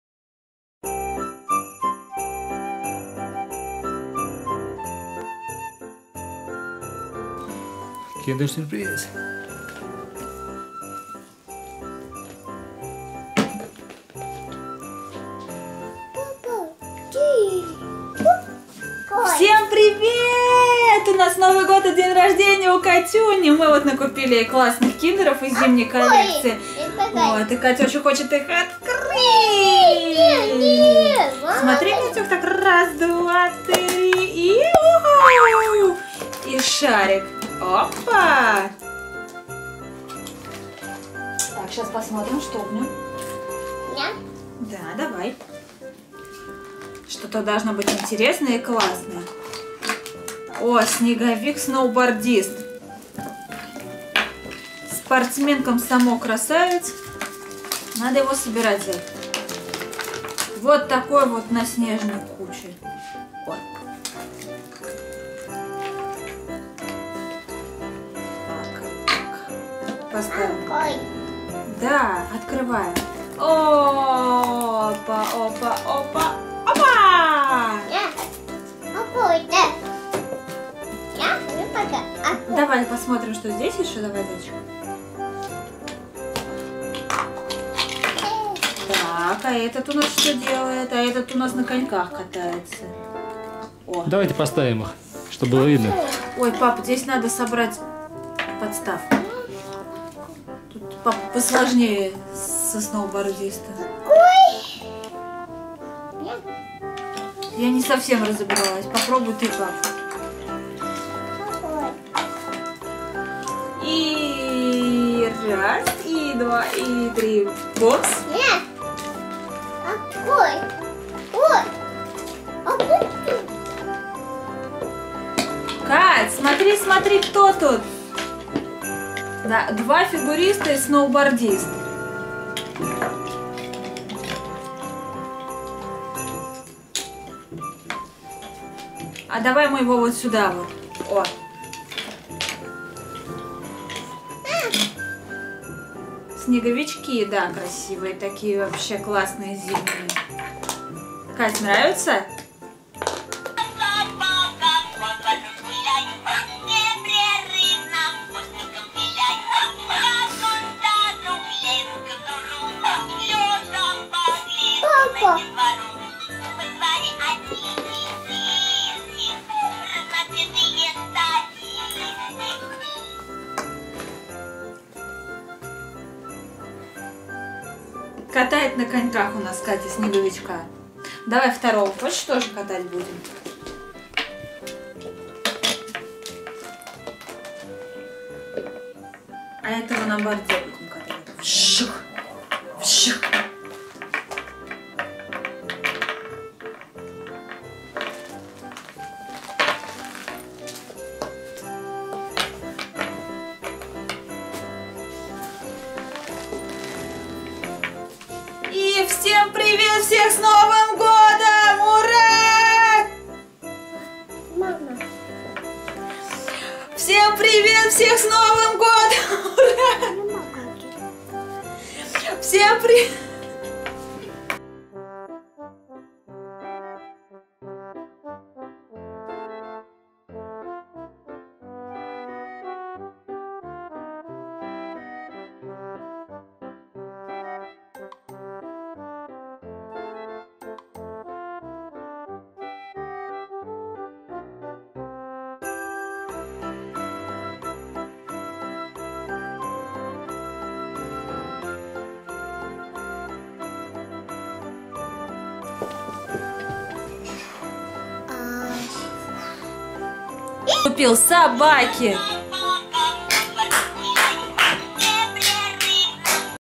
День рождения Кейт. Начало. Катя распаковывает киндер сюрпризы макси.
День рождения Кейт. Начало. Киндер макси. Всем привет! Поздравляем всех с Новым годом! У нас еще праздник. День рождения Кейт. Мы открываем три киндер сюрприза макси из зимней коллекции. Завтра у нас детская вечеринка: будем веселиться, соберем автотрек Magic Tracks, поиграем с машинкой молния маквин и многое другое.
Наступил 2018 год! Ура! Будем праздновать день рождения Кати!

В продолжение дня рождения Кати будут новые интересные видео со встреч с друзьями!